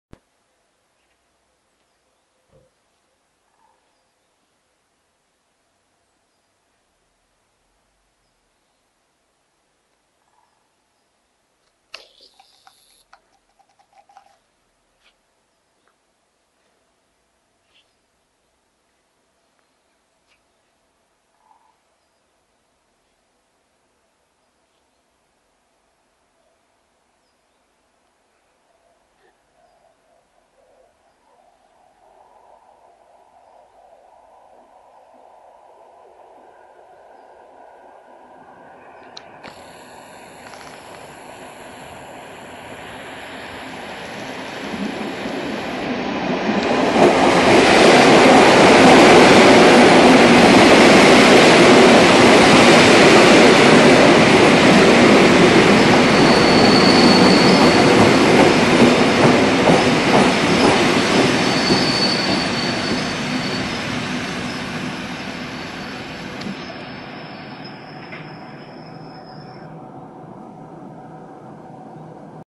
EN57AKM-1299+EN57AKM-1578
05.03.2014. EN57AKM o poranku wjeżdżają na przystanek Świder jako pociąg osobowy KM 705 z Warszawy Zachodniej do Otwocka.